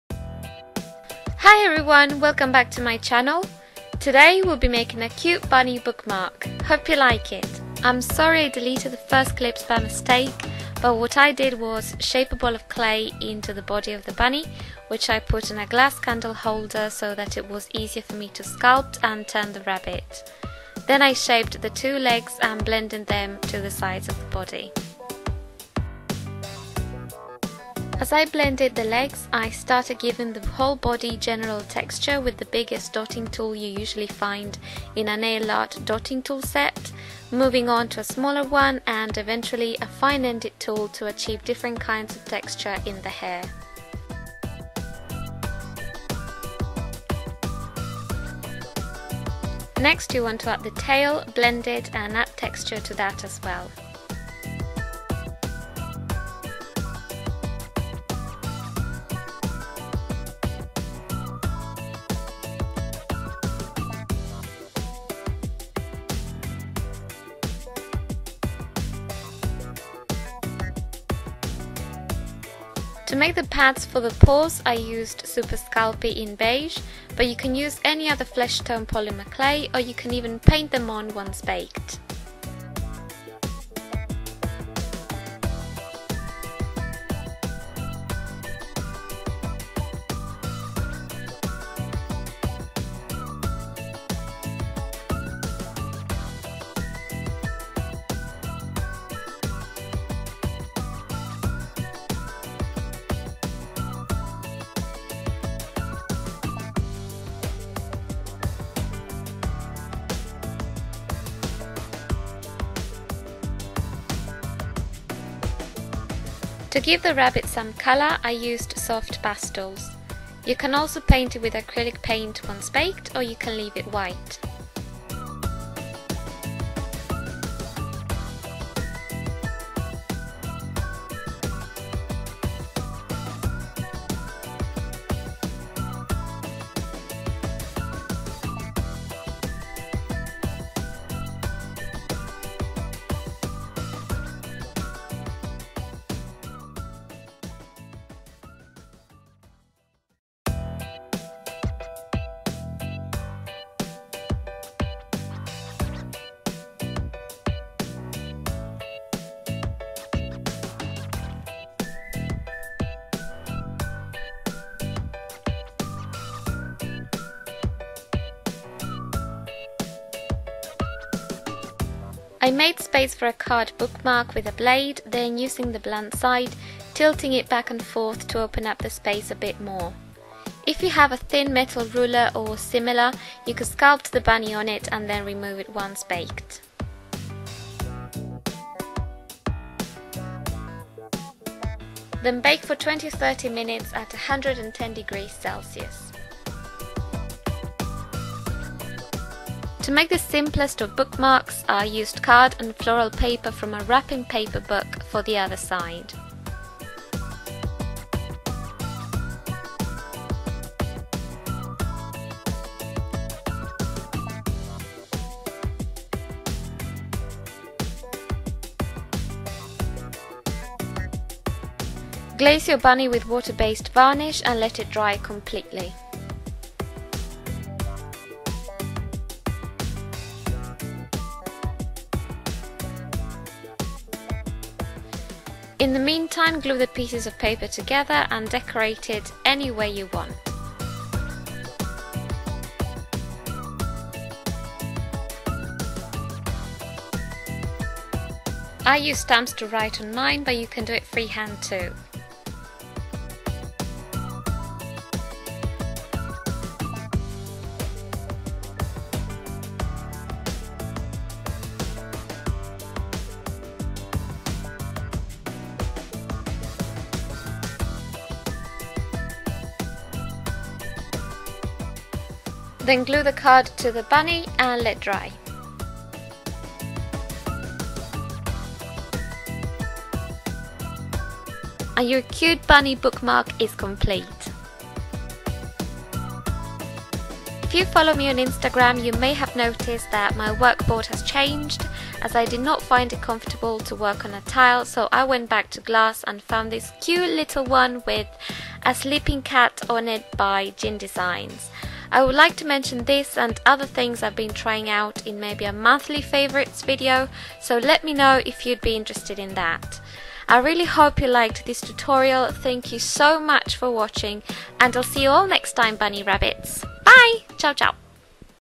Polymer Clay Cute Rabbit Bookmark Tutorial | Maive Ferrando
Hello, I'm Maive. In this video I'll be showing you how I made a cute bunny feet bookmark using polymer clay.

🍓 ETSY SHOP

- Card
- Wrapping /Scrapbooking paper
- Stamps
- Bake at 110ºC for 20-30 minutes.